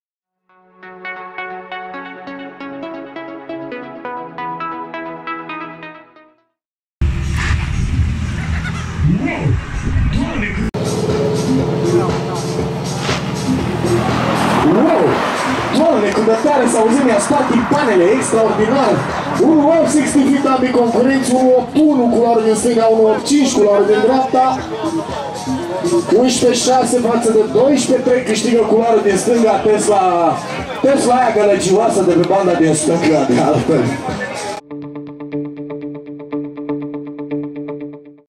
Tesla Model 3 Performance 11.6 vs Tesla S P85D 12.3 Drag Race
Drag Racing Turda 2019
402m